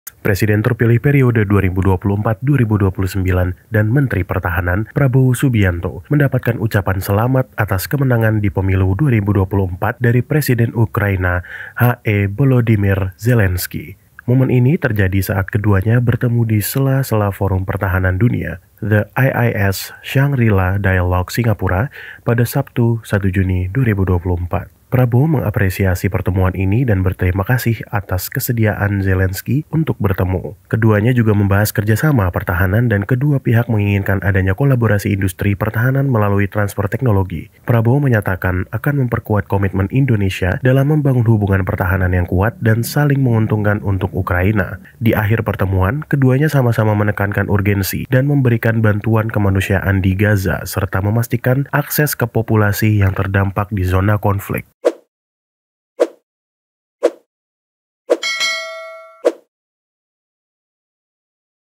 Prabowo Bertemu Zelenskyy, Ini Yang Di Bahas
Presiden terpilih periode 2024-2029 dan Menteri Pertahanan, Prabowo Subianto, mendapatkan ucapan selamat atas kemenangan di pemilu 2024 dari Presiden Ukraina H.E Bolodymyr Zelenskyy. Momen ini terjadi saat keduanya bertemu di sela sela Forum Pertahanan Dunia, The Iiss Shangri La Dialogue Singapura, pada Sabtu 1 Juni 2024. Prabowo mengapresiasi pertemuan ini dan berterimakasih atas kesedian Zelensky untuk bertemu. Keduanya juga membahas kerja sama pertahanan dan kedua pihak menginginkan adanya kolaborasi industri pertahanan melalui transfer teknologi. Prabowo menyatakan akan memperkuat komitmen Indonesia dalam membangun hubungan pertahanan yang kuat dan saling menguntungkan untuk Ukraina. Diakhir pertemuan keduanya sama sama menekankan urgensi, dan menberikan bantuan kemanusiaan di Gaza serta ke populasi yang terdampak di zona konflik.

Ikuti juga update berita selengkapnya di YouTube Berita Nasional TV dan Website Berita di Berita Nasional.com